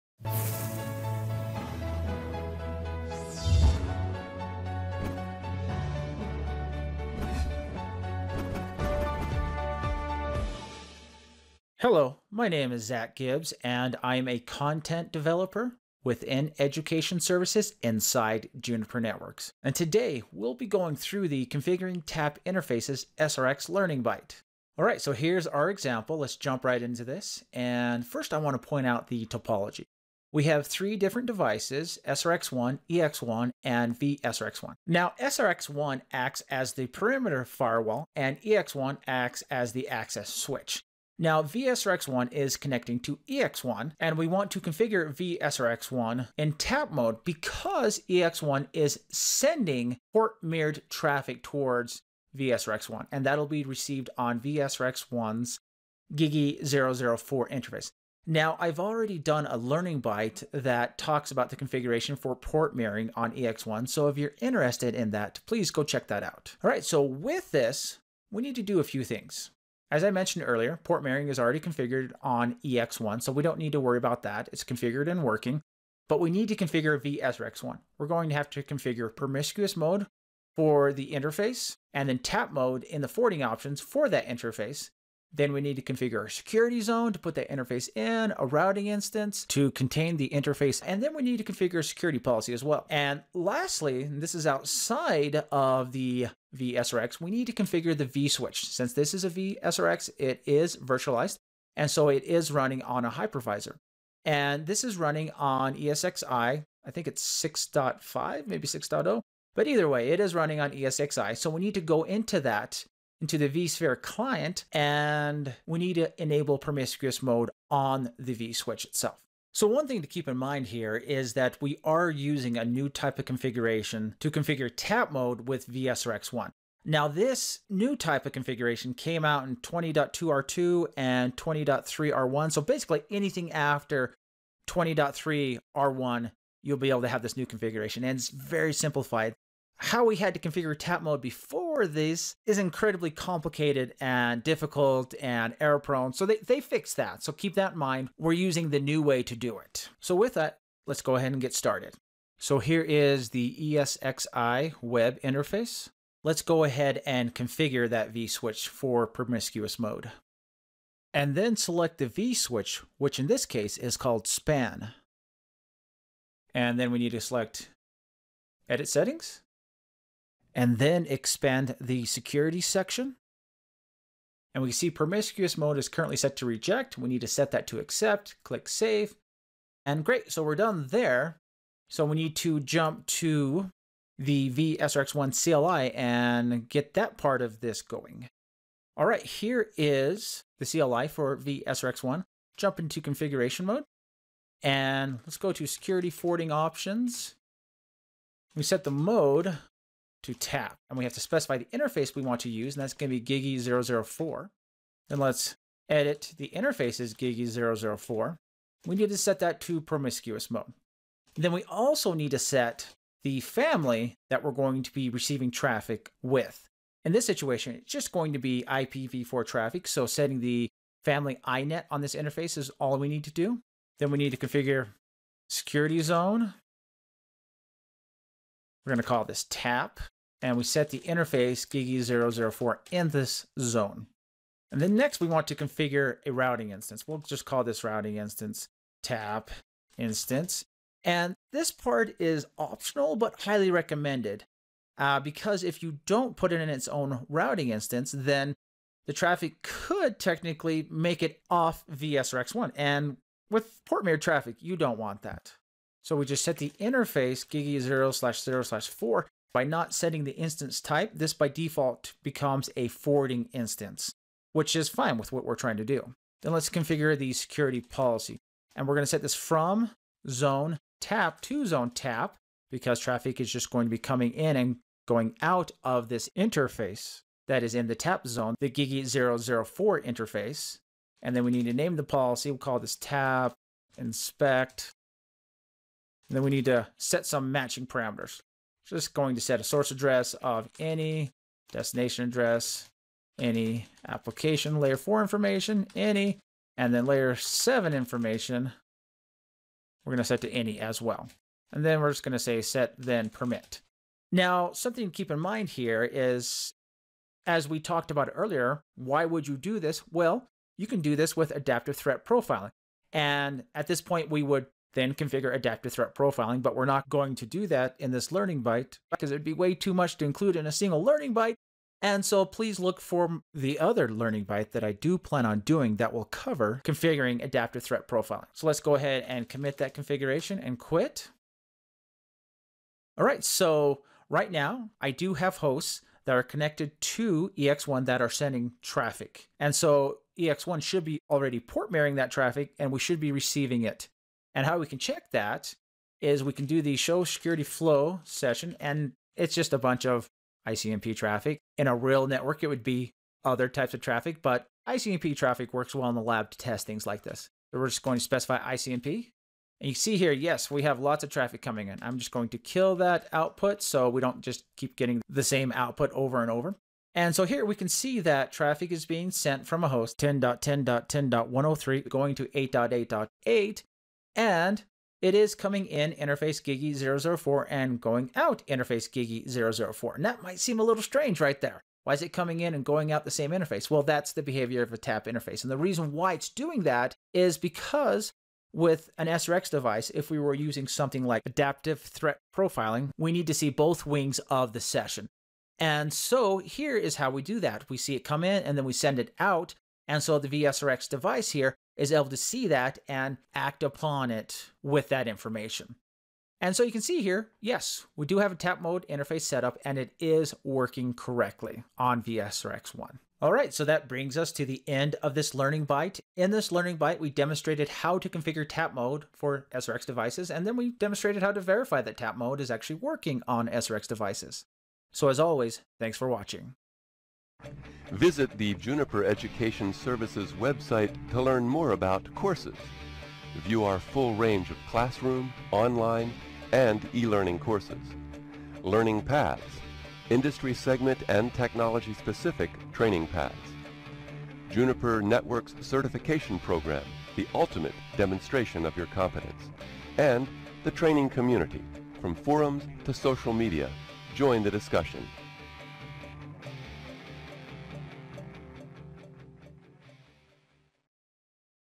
Configuring Tap Interfaces: SRX Series
The Configuring Tap Interfaces – SRX Series Learning Byte covers how to configure tap interfaces on Juniper SRX Series devices. This Learning Byte is most appropriate for users who are familiar with SRX Series devices.
• Presenter: Zach Gibbs / Content Developer

• Relevant to OS Releases: Junos 20.3R1 or later

• Relevant to Juniper Platforms: SRX Series